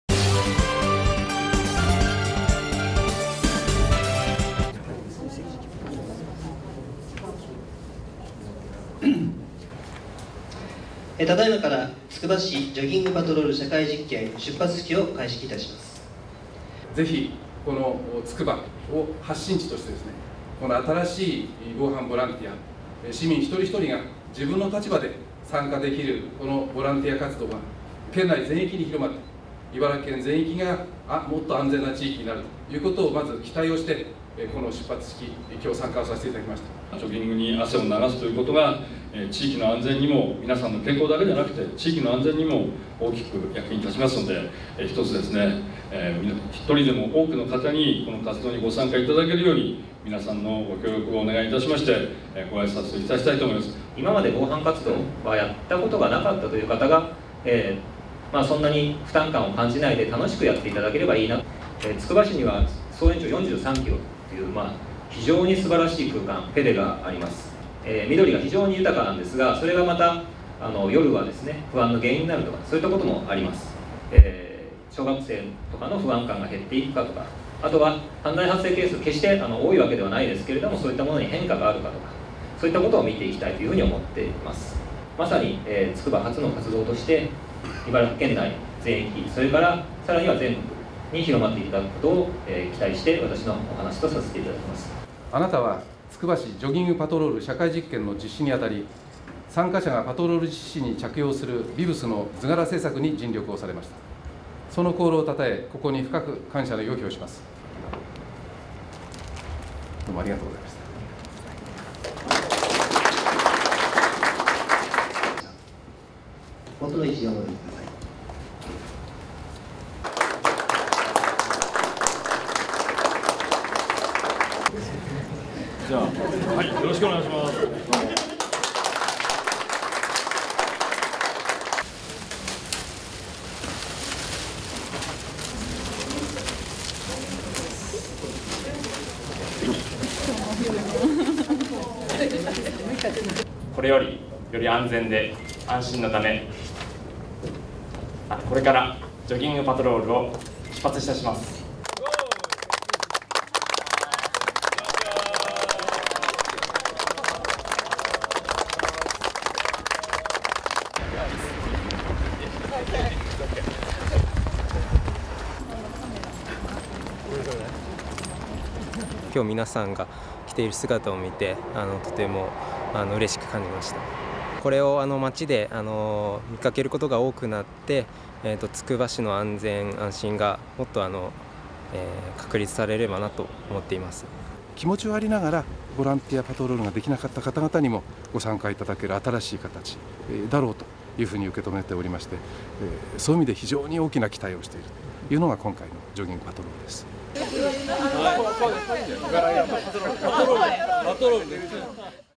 走って防犯活動 つくばで社会実験
健康志向の高まりでマラソンブームが続く中、県警とつくば市は11日、ジョギングと防犯パトロールを合体させた県内初の社会実験をスタートさせた。「ジョグパト」の愛称で、「見てるよ」と書かれたビブスをジョギング愛好家に着用してもらい、地域の犯罪抑止につながるか検証する。メンバーの固定化など組織運営で悩む防犯ボランティア団体が多い中、県警は「誰でも無理なく参加できる防犯活動」として、今後は他の自治体にも普及させたい考え。

　実験は、総延長43キロの歩行者専用道路がある中心市街地を重点地域とし、市内全域を対象とした。ビブスは２色あり、市のマスコットキャラクター、フックン船長が監視の目を光らせる絵柄をプリント。現在、市が窓口となり、１８歳以上で参加者（定員２００人）を受け付けている。

　参加者は、子どもたちの登下校の時間帯などに街中を走り、不審者がいれば１１０番通報する。実験の検証期間は来年３月まで。その間、アンケートを通じて、市民の犯罪不安の低減につながるかなどを調べる計画。

　同日、つくば市役所で出発式が行われ、ジョギング愛好者人にビブスが手渡された。

　ジョグパト発案者で、防犯まちづくりが専門の樋野公宏東大大学院准教授（３９）は「ジョギングは愛好者が多い上、広い範囲を網羅する機動性がある。人の目を増やすことは重要で、モデル地区となったつくば市からこの活動を全国に広めたい」と語った。